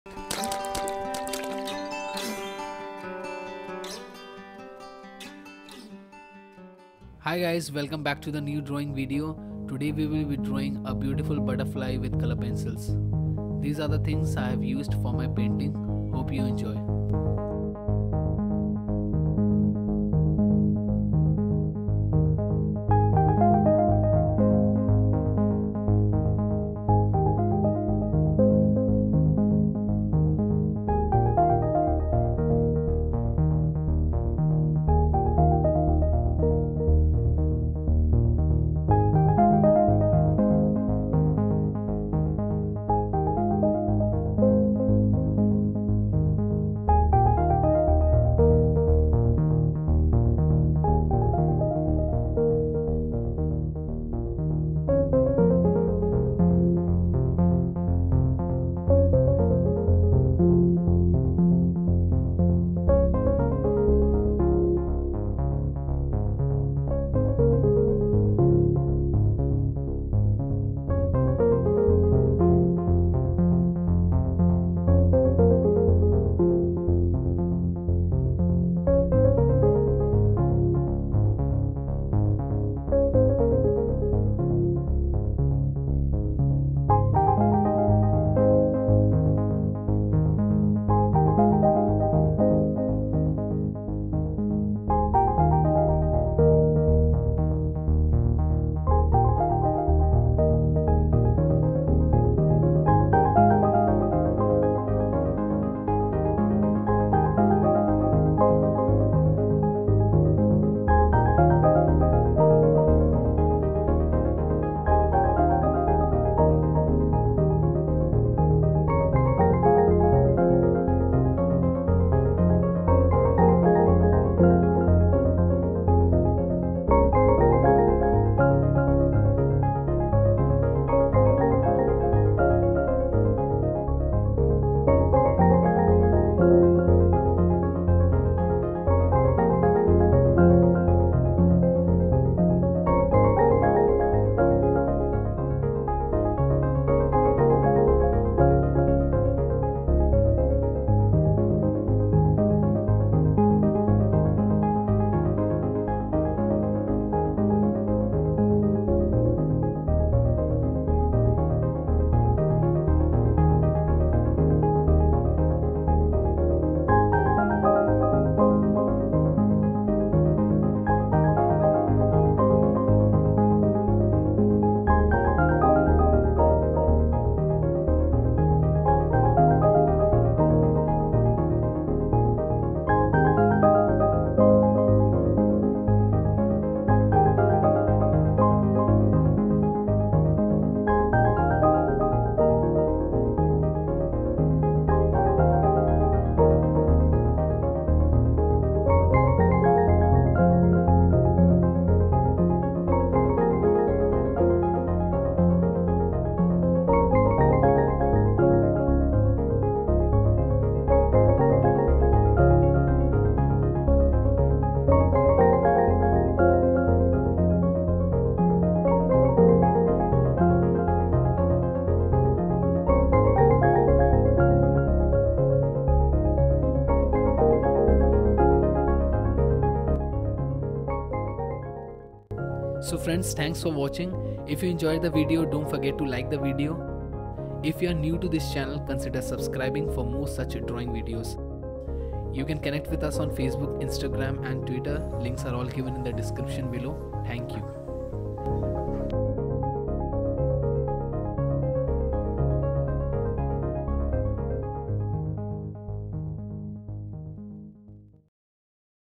How to draw a butterfly with colored pencils - Pencil drawing
In this video, I have drawn a butterfly with colored pencils. Pencil drawing. This is a simple pencil drawing with coloured pencils of a butterfly. For this painting, I have used all the materials specified below.This is a pencil sketch drawing. hope you enjoy this coloured pencil drawings...

                       Things used:-

1. Faber Castell Polychromos pencils
2. Ivory drawing sheet
3. normal and kneaded eraser
4. normal HB pencil
5. white pen ( Uniball Signo )